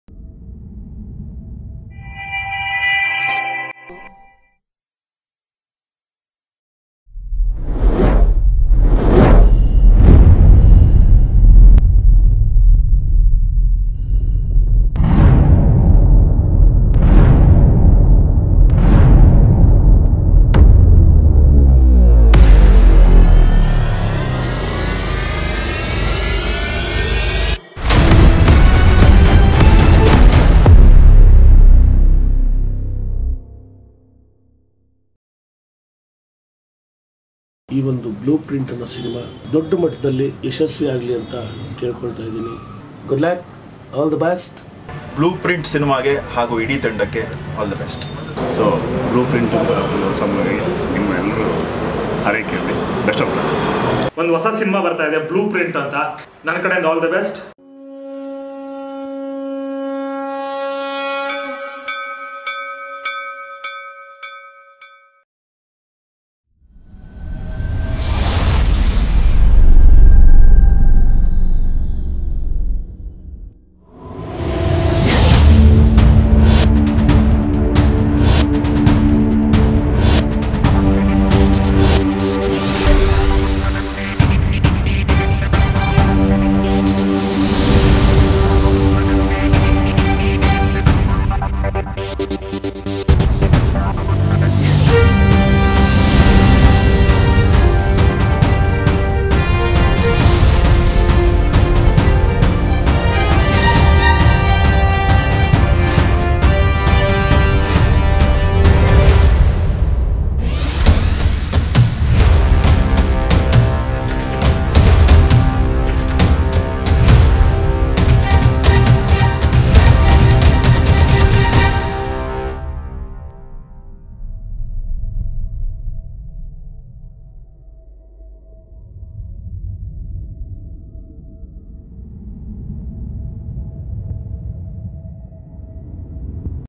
Upendra Luanch Blue Print Movie 1st look, Darshan, Sudeep, Yash & All Fans Made the Movie
Upendra sir Luanch Blue Print Kannada Movie 1st look, Aryan Sira, Darshan, Sudeep, Yash & All Fans Made the Movie
Chakravarti Sulibele Heart touching Speech | ಚಕ್ರವರ್ತಿ ಸುಲಿಬೆಲೆ ಹೃದಯ ಸ್ಪರ್ಶಿ ಭಾಷಣ